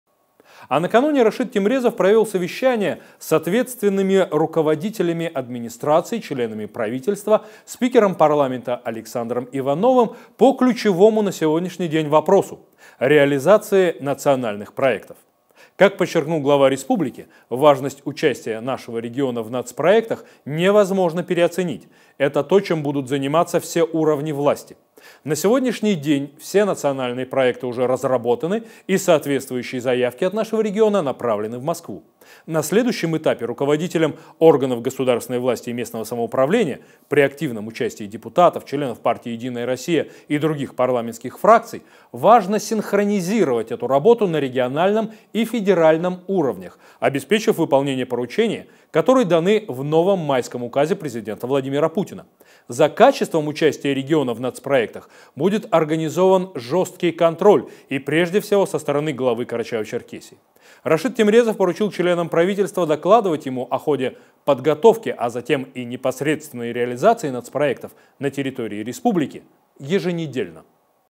Рашид Темрезов держит под личным контролем участие КЧР в реализации Национальных проектов
Рашид Темрезов провел совещание с ответственными руководителями Администрации, членами Правительства, спикером Парламента Александром Ивановым по ключевому на сегодняшний день вопросу – реализации Национальных проектов. Как подчеркнул глава республики, важность участия нашего региона в Нацпроектах невозможно переоценить - это то, чем будут заниматься все уровни власти. На сегодняшний день все национальные проекты уже разработаны и соответствующие заявки от нашего региона направлены в Москву. На следующем этапе руководителям органов государственной власти и местного самоуправления при активном участии депутатов, членов партии «Единая Россия» и других парламентских фракций, важно синхронизировать эту работу на региональном и федеральном уровнях, обеспечив выполнение поручений, которые даны в новом «Майском» Указе Президента Владимира Путина. За качеством участия региона в Нацпроектах будет организован жесткий контроль и прежде всего со стороны Главы Карачаево-Черкесии. Рашид Темрезов поручил членам Правительства докладывать ему о ходе подготовки, а затем и непосредственно о реализации Нацпроектов на территории республики еженедельно.